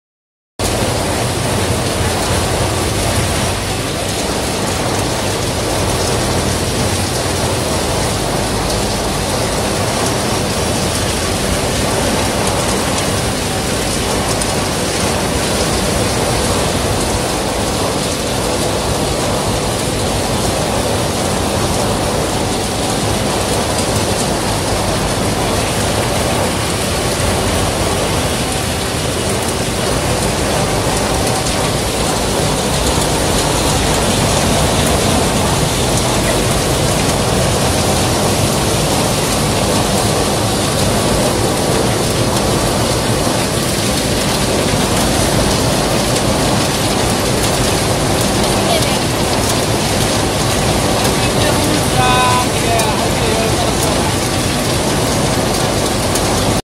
Working video of the ring die pellet line
The working plant is made by our E.P Machinery. The raw materials for the pellet production line is sawdust. The capacity for the line is 2ton/hour with two sets 420 ring die pellet mill.